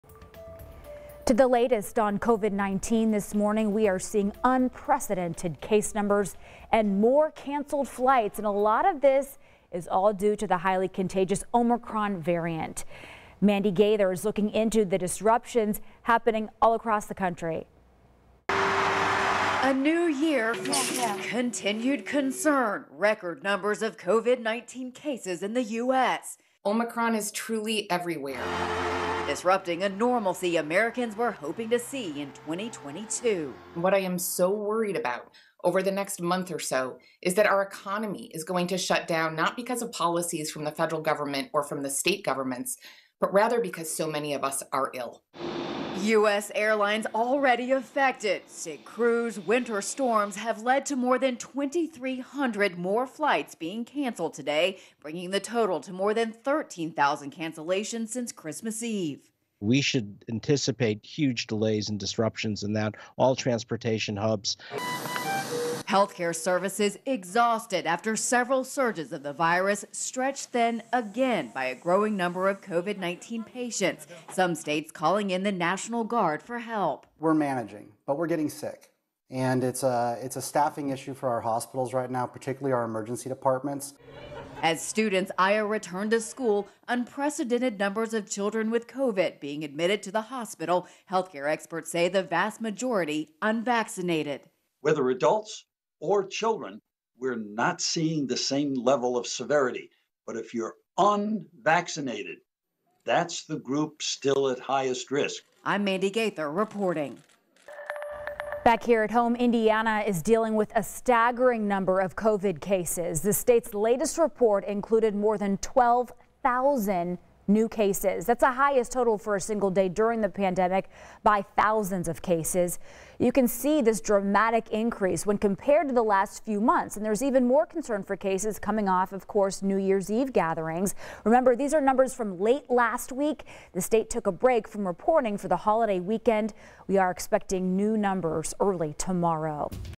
Sunday morning COVID-19 update: Jan. 2, 2022
Sunday morning's latest update on the coronavirus pandemic for Jan. 2, 2022.